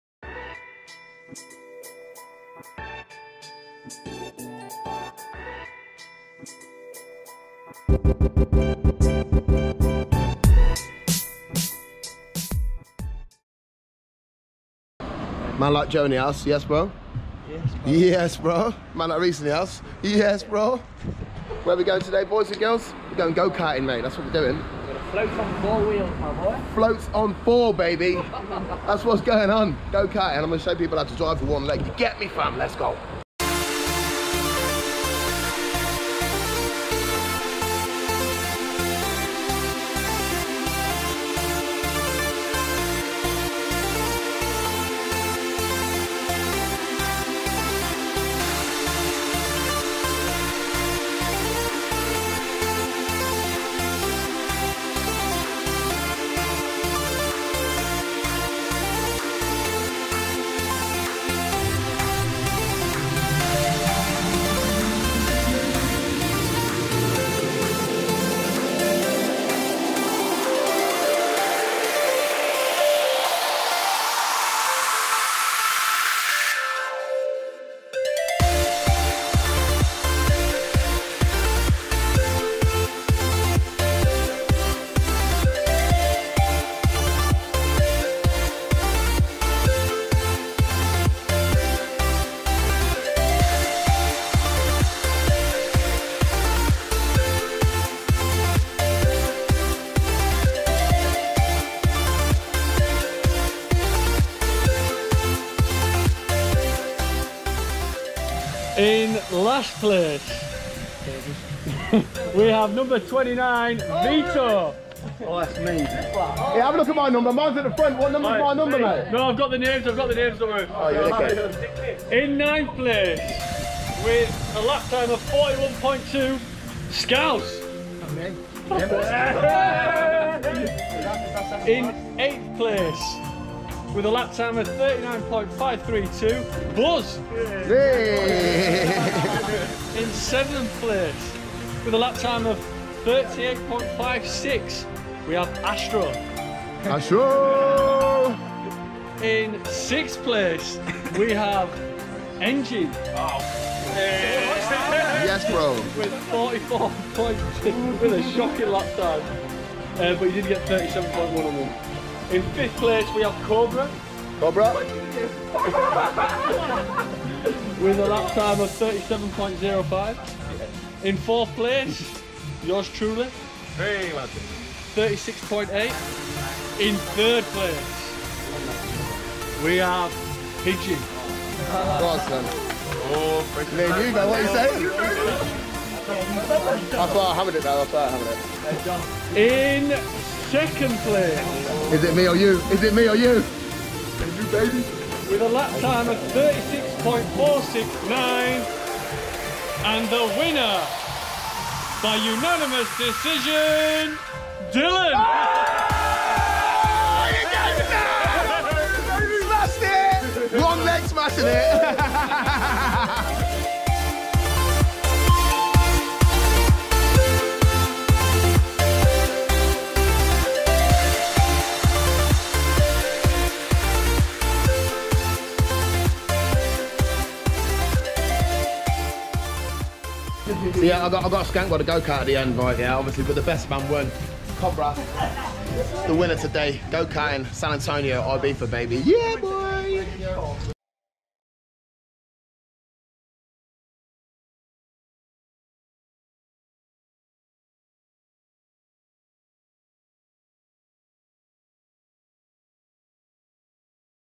An Amputee Go Karting
Today we take a trip to a Go Karting track with the lads from Linekers bar Ibiza.